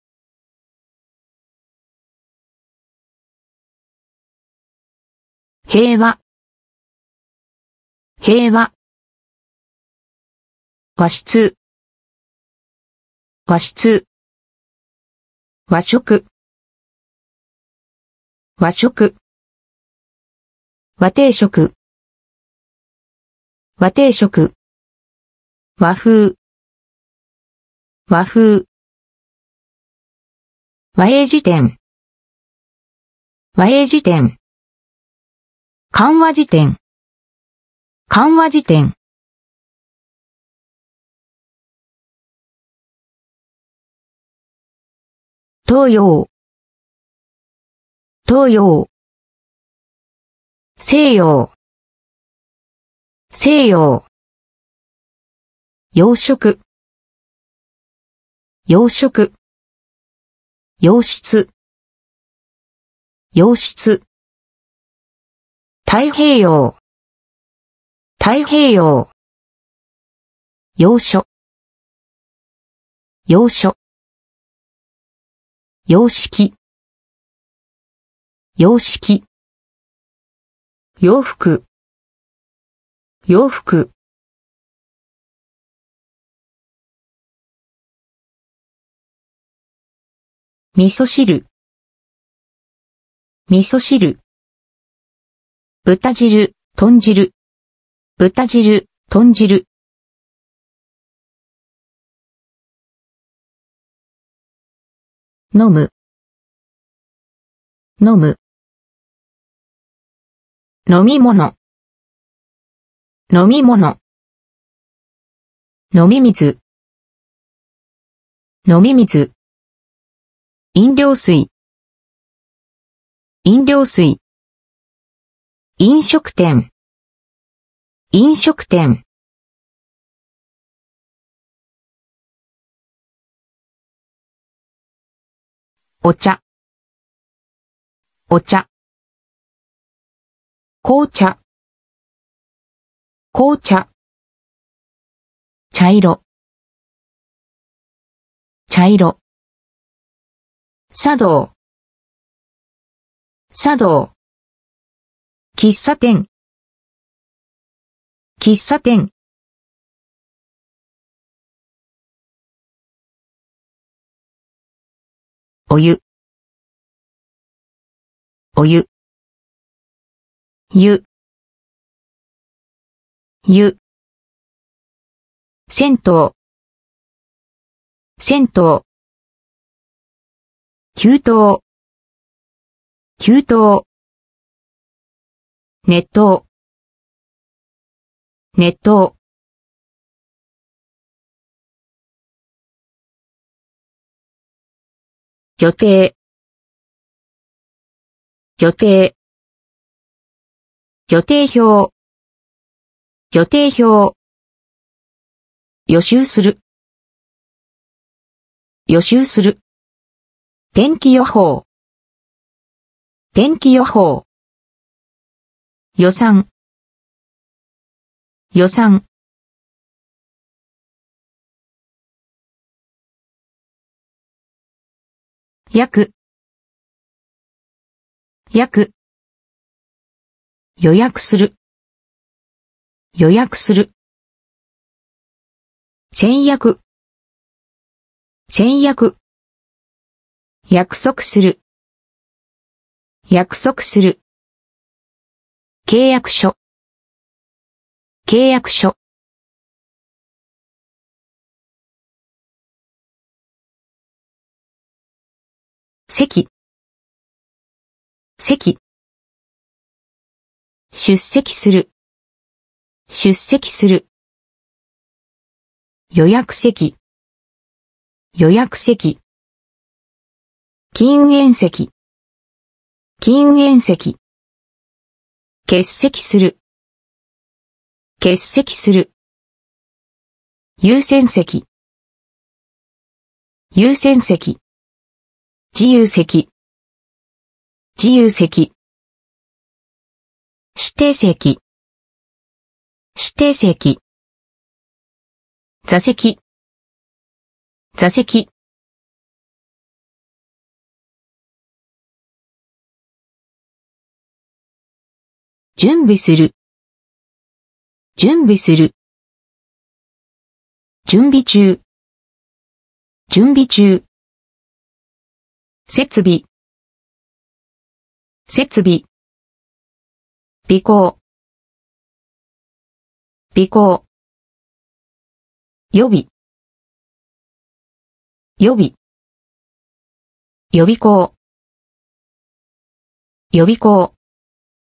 250 Essential Kanji for Everyday Use, Lesson 20: Reading Restaurant Signs and Menus
Kanji: 和 洋 汁 飲 茶 湯 予 約 席 備